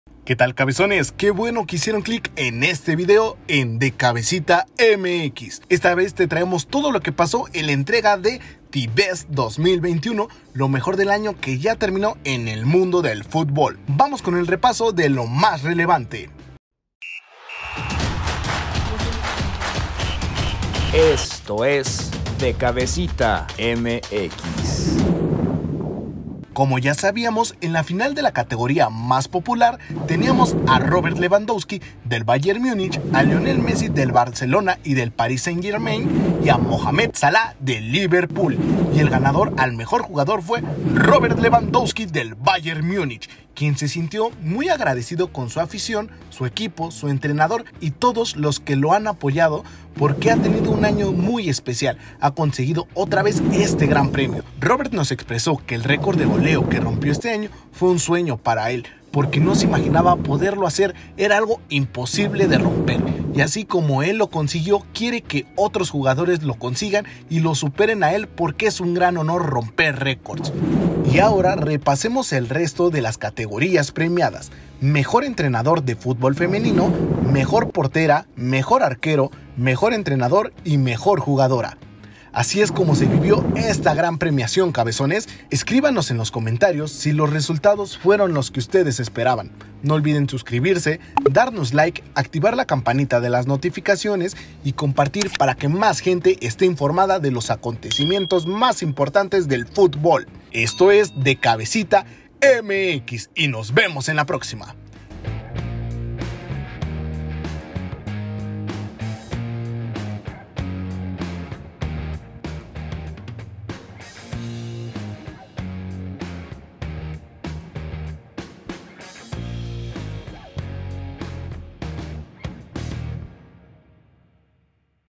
🏆FIFA THE BEST 2022 ESPAÑOL😱 GANADORES
Esto fue lo más relevante que sucedió durante la Ceremonia The Best 2022 de la FIFA.